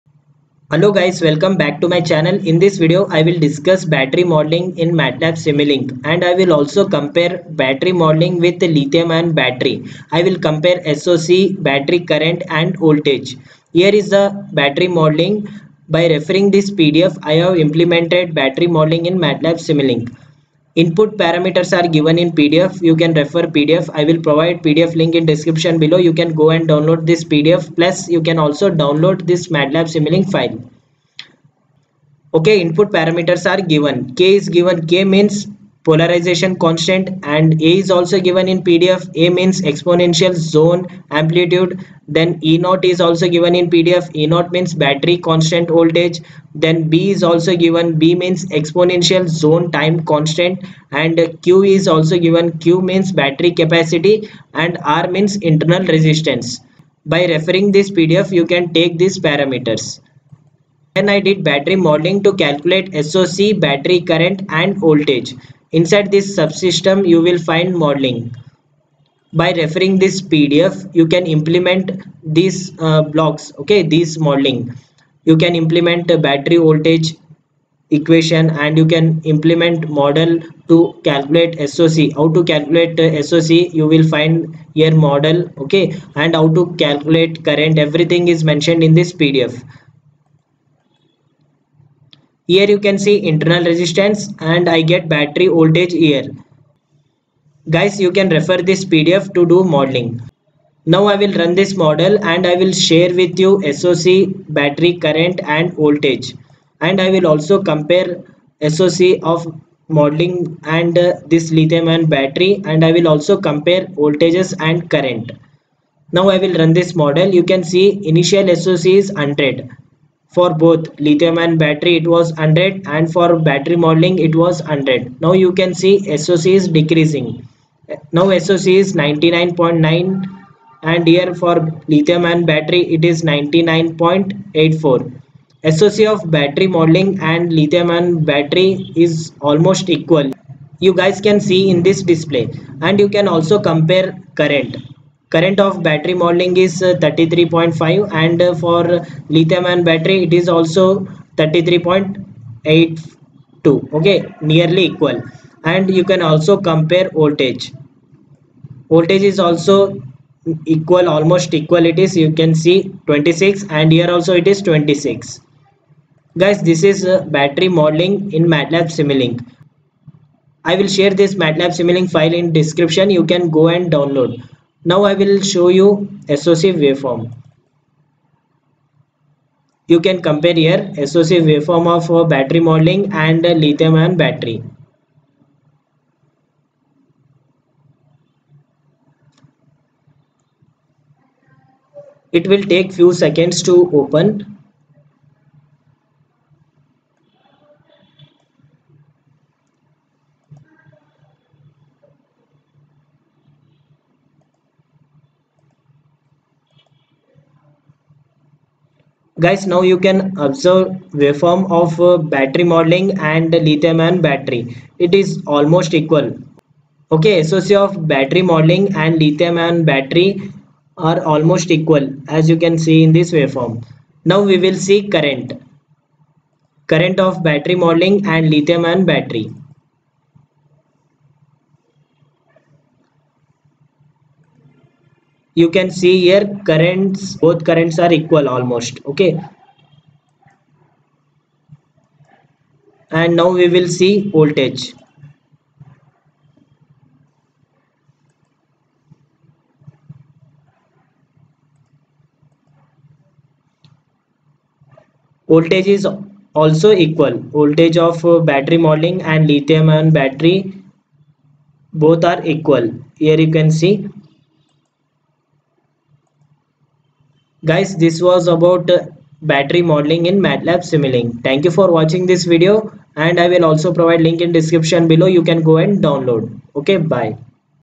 Battery Mathematical Modeling In MATLAB Simulink, Download MATLAB File
In this video, I will discuss lithium ion battery Mathematical Modeling In Matlab Simulink.

My website's -